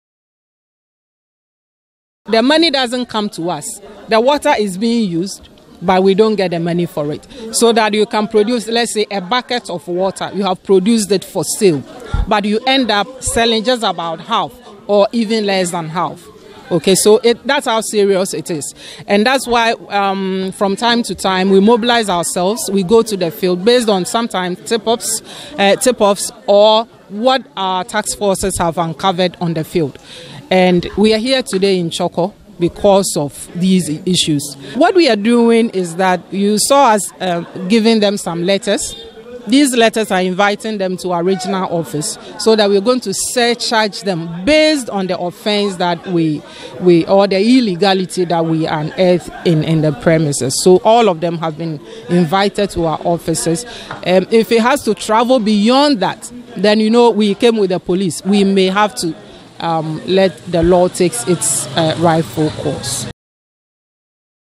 GWCL cuts water supply to over 20 houses at Chorkor due to illegal connections
The Ghana Water Company Limited (GWCL) has cut supply to over 20 houses at Chorkor due to illegal connections.